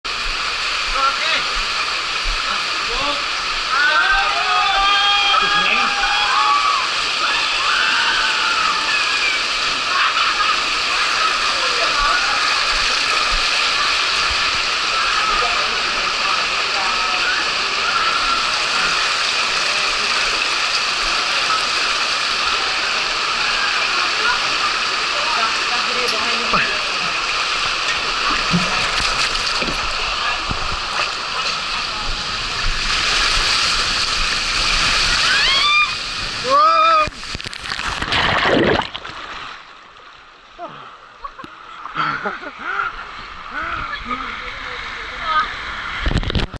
Rainbow Racer Slide at Ocean Park Vientiane
Rainbow Racer water park slide at Ocean Park Vientiane aqua park in Vientiane, Laos.